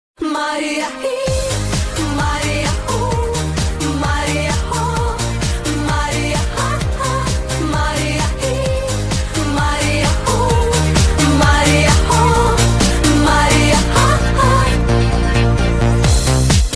Miniature Pinscher cleaning assistant
My cleaning assistant with great motivation.
Model: Min Pin Peremoga TheIslandOfTreasureskennel ,5 месяцев